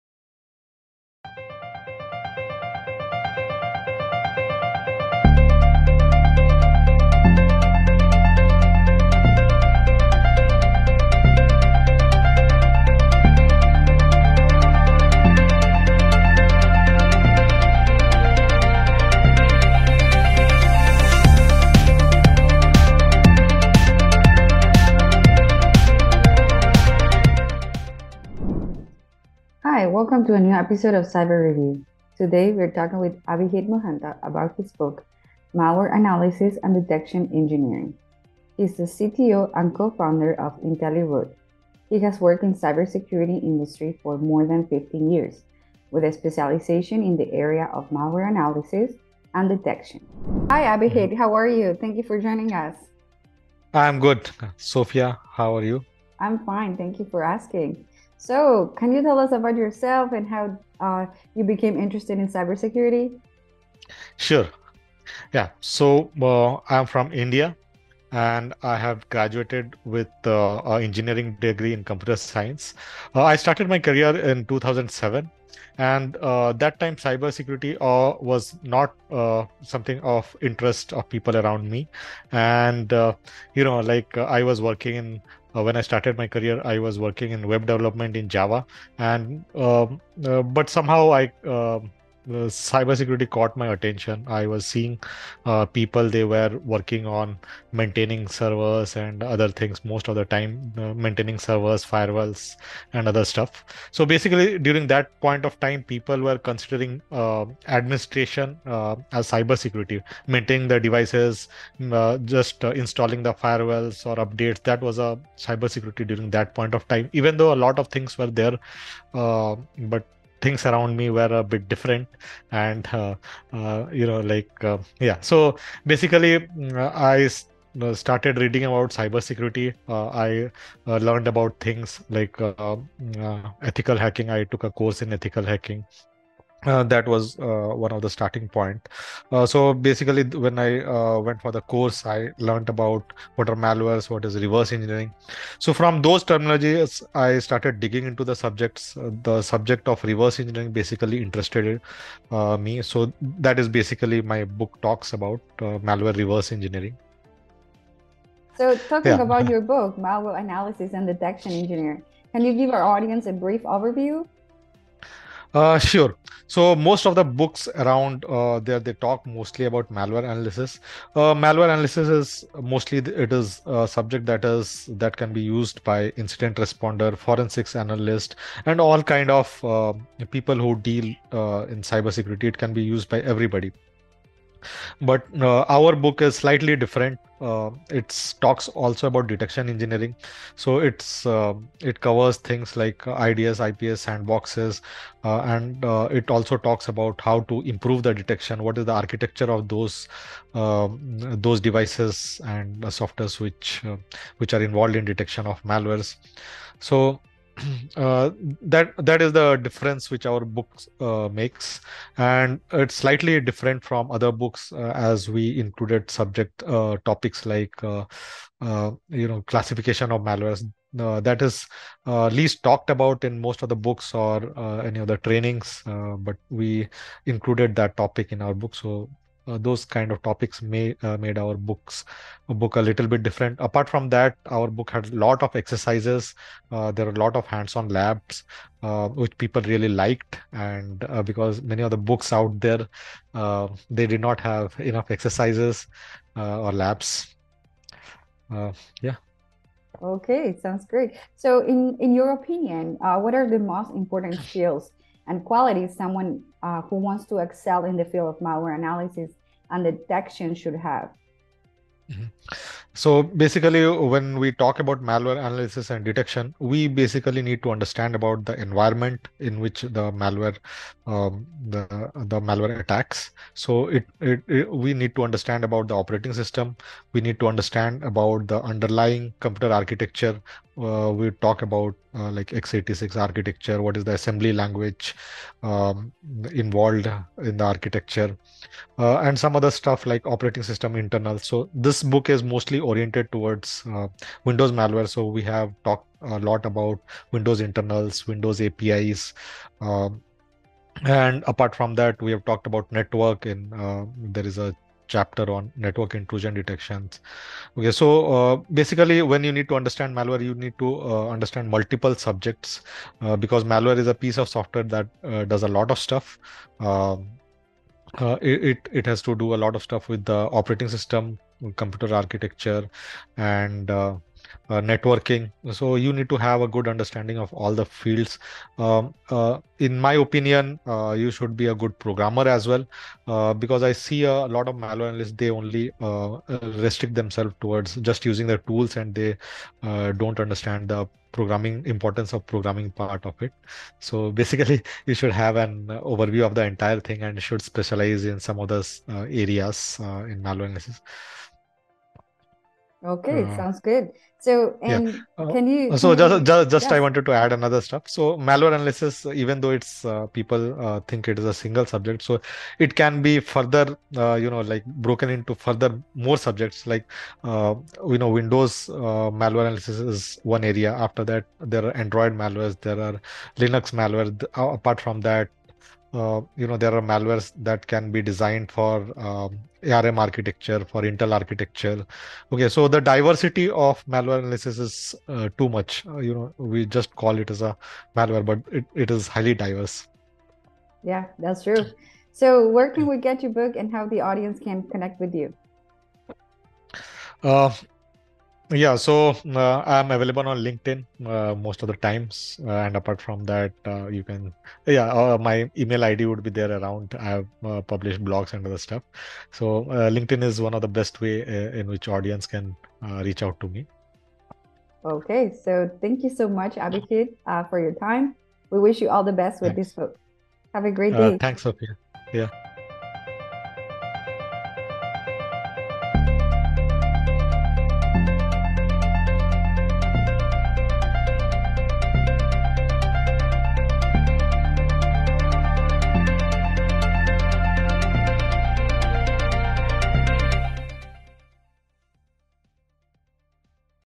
Cyber Review: Malware Analysis by Abhijit Mohanta
Today, we’re talking with  Abhijit Mohanta  author of the book  Malware Analysis and Detection Engineering: A Comprehensive Approach to Detect and Analyze Modern Malware

He is the CTO and Co-Founder of Intelliroot. He has worked in the Cybersecurity Industry for 15+ years with specialization in the area of Malware Analysis and Detection and has several patents in the field